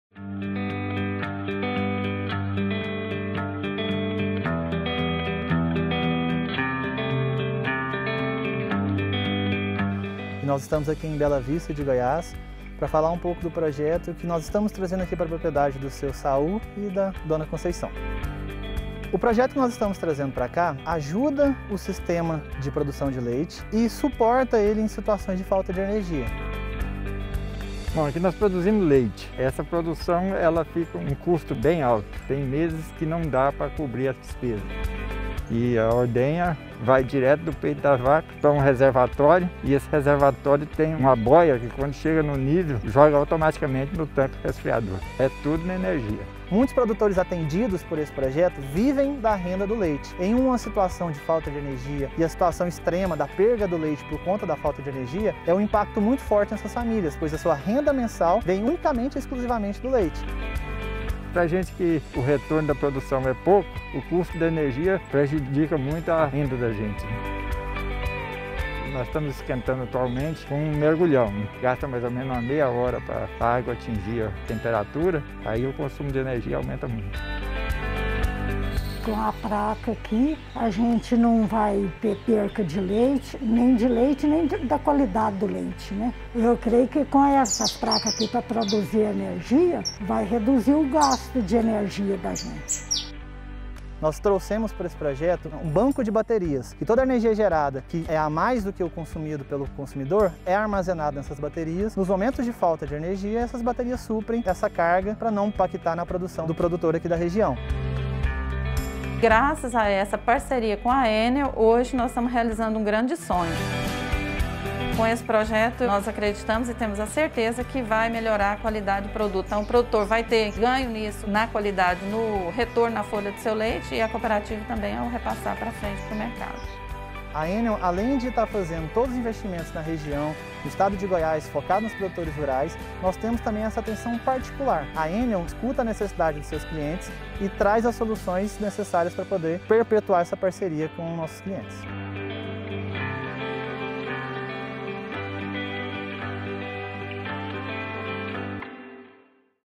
EnelPorGoiás | Projeto com produtores rurais para instalação de placas solares
Estamos realizando um piloto com 28 produtores rurais goianos de instalação de sistemas de geração de energia solar fotovoltaica.